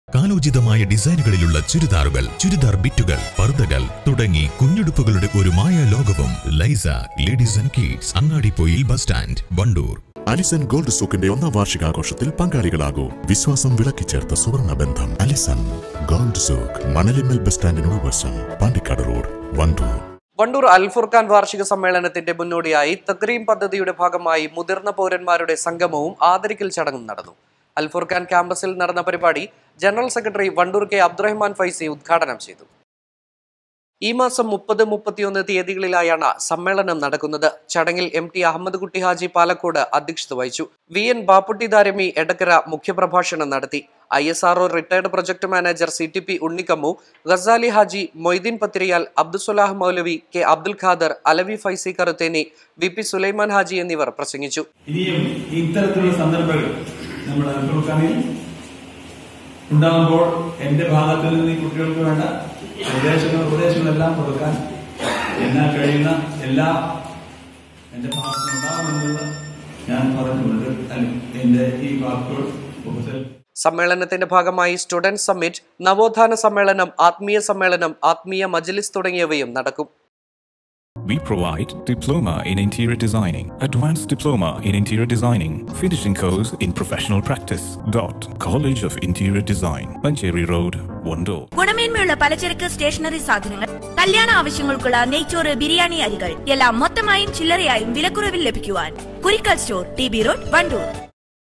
വണ്ടൂർ അൽഫുർഖാൻ വാർഷിക സമ്മേളനത്തിന്റെ മുന്നോടിയായി മുതിർന്ന പൗരന്മാരുടെ സംഗമം നടന്നു
വണ്ടൂർ അൽഫുർഖാൻ വാർഷിക സമ്മേളനത്തിന്റെ മുന്നോടിയായി 'തക് രീം' പദ്ധതിയുടെ ഭാഗമായി മുതിർന്ന പൗരന്മാരുടെ സംഗമവും ആദരിക്കൽ ചടങ്ങും നടന്നു